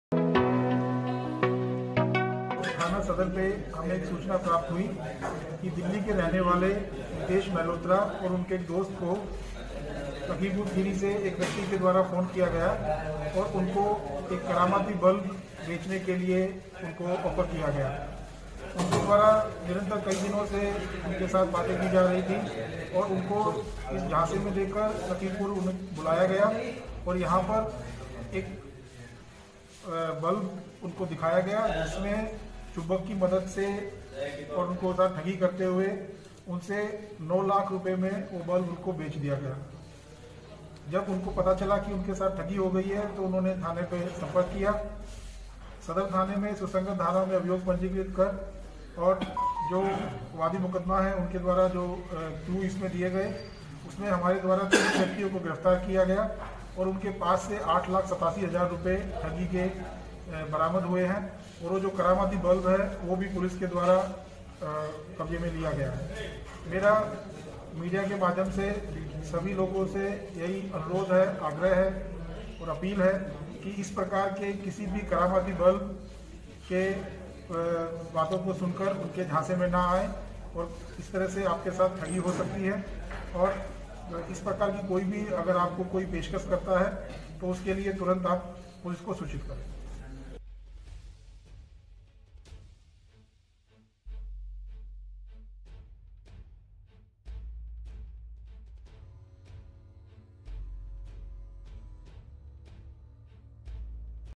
Delhi के व्यापारी को UP में बेचा 9 लाख का साधारण बल्ब, बताई सोने-चांदी जैसी कीमती चीज़े मिलेंगी
कुछ ठगों द्वारा दिल्ली के एक व्यापारी को 9 लाख रुपये में ‘जादुई बल्ब’ बेचने का मामला सामने आया है. ठगों ने व्यापारी को ऐसी पट्टी पढ़ाई कि इस बल्ब से उसे सोने-चांदी जैसी कीमती धातुएं मिलेंगी और घर में समृद्धि आएगी. लेकिन वास्तव में वो एक साधारण बल्ब निकला...